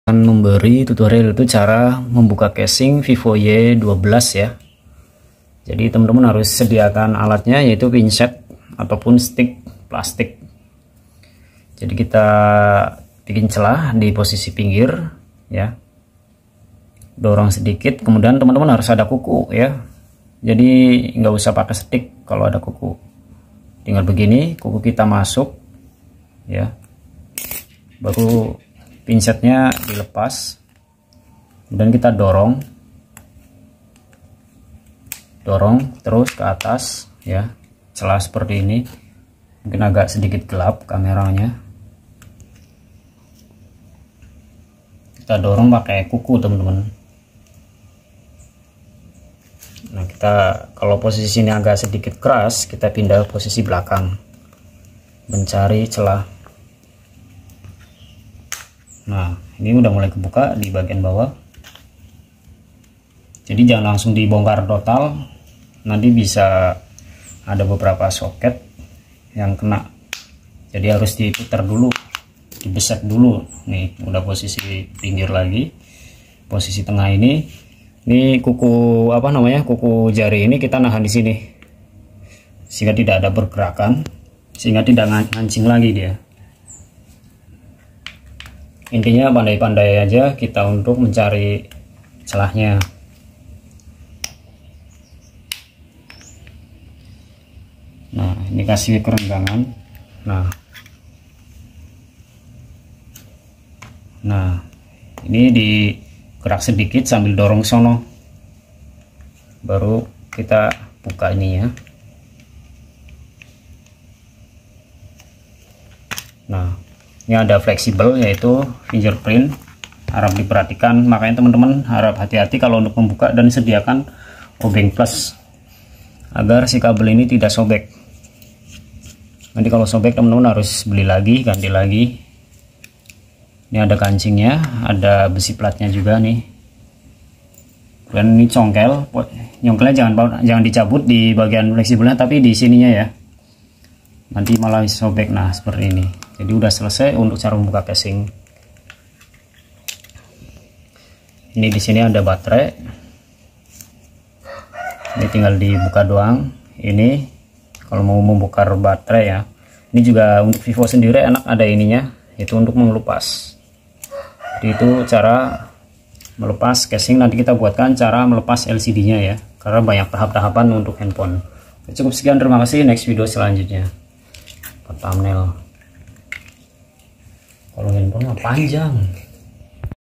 Cara membuka casing hape VIVO Y12
Tutorial cara membuka atau melepas casing hape vivo y12
Video singkat tips membuka kesing hape merk vivo y12 dengan mudah dan praktis.

chek out hape Vivo y12 di Shopee
Follow Social Media Wong Pruwatan

Aja Klalen!